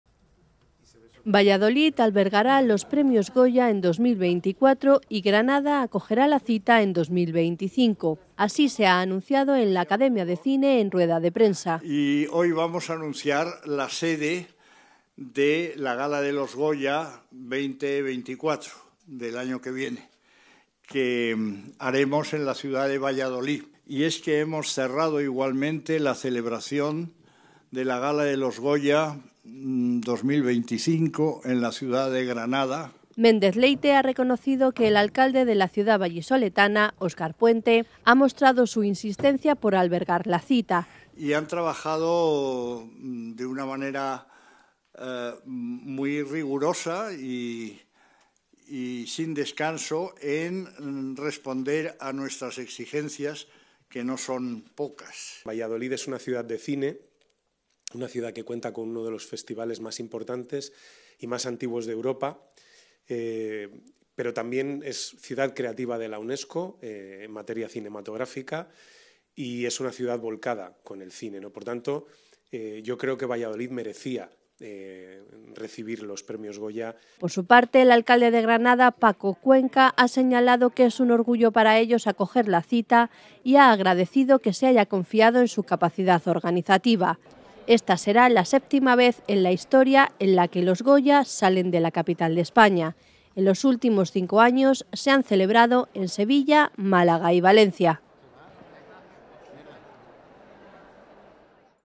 Valladolid acogerá los premios Goya en 2024 y Granada en 2025
La Academia de Cine ha anunciado que Valladolid acogerá la 38 edición de los Premios Goya, según ha desvelado el presidente de la institución, Fernando Méndez-Leite en rueda de prensa, y posteriormente ha anunciado que Granada albergará la 39 edición de los Premios Goya en 2025.